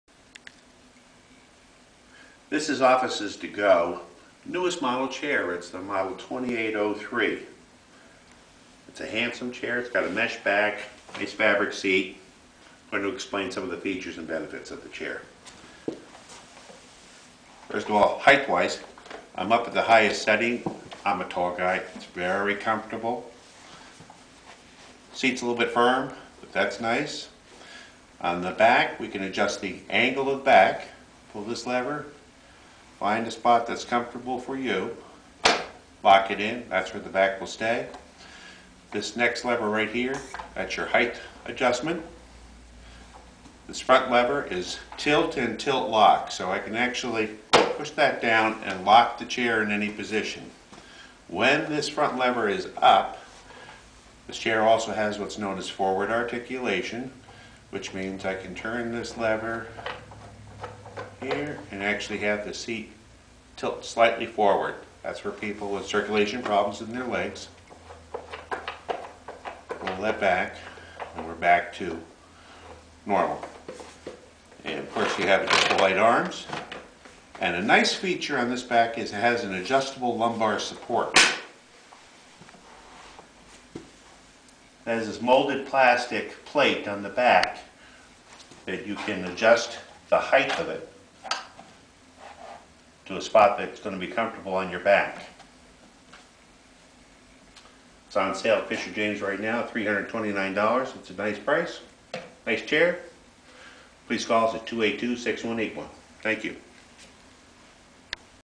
Offices to Go 2803 4x4 Multi-Function Chair presented by Fisher James Furniture Company
Robert Hayward shows the Offices to Go 2803 4x4 Multi-Function Chair features.